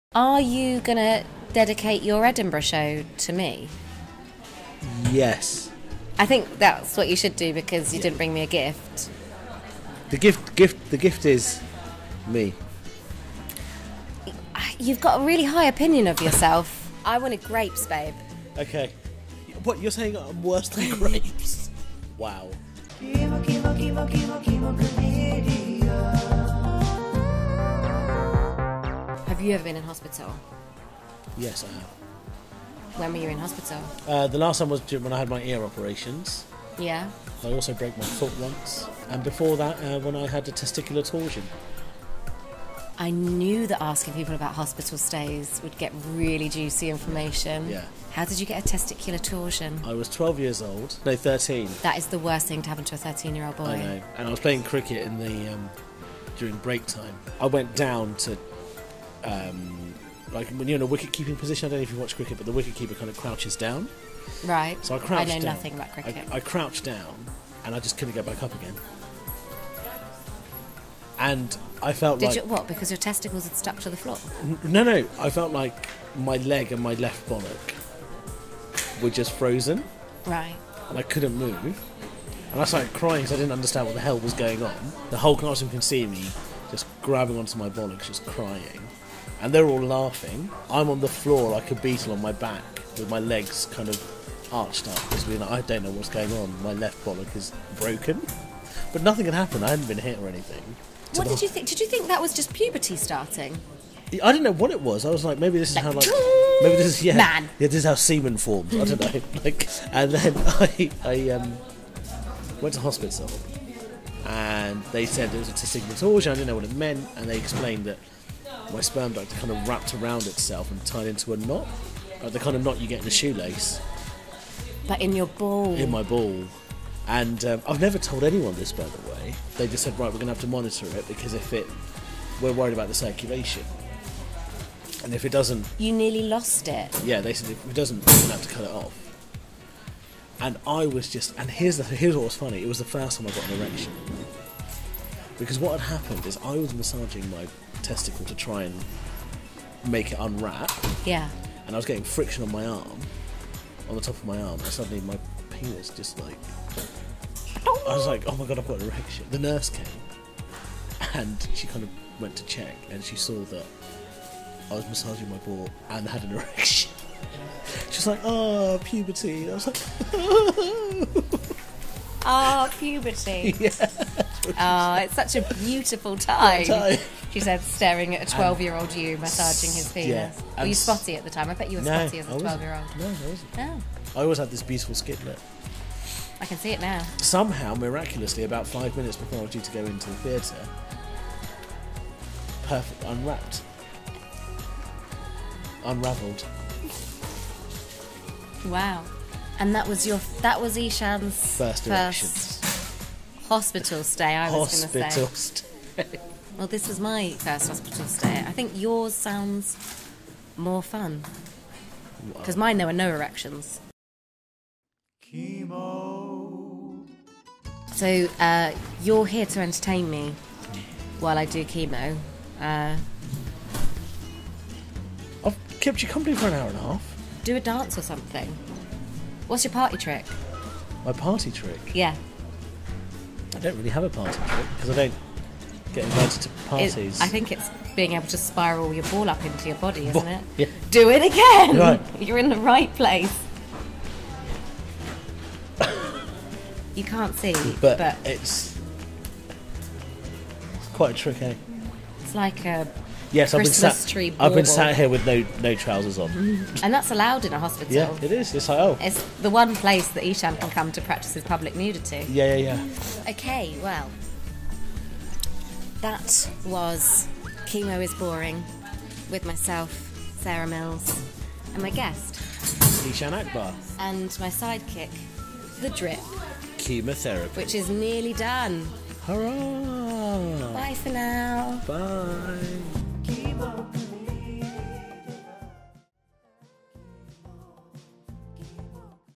The Chemo Chat Show....with Eshaan Akbar (Pt 5)
The world’s ONLY chat show to broadcast from a chemo ward…

...this week, we’re talking TESTICULAR TORSION.

I've got to thinking a lot about how our bodies embarrass us while I've been ll. Everyone has great stories about excruciating hospital trips and Eshaan is no exception….

Edited by Jack Charles Bushell. Thanks to Alasdair Beckett-King, Friz Frizzle and of course the apple of my eye Eshaan Akbar.

(If you have any worries about your own health, or if you’d like to support them, Macmillan’s website is and more info and support regarding Bowel Cancer can be found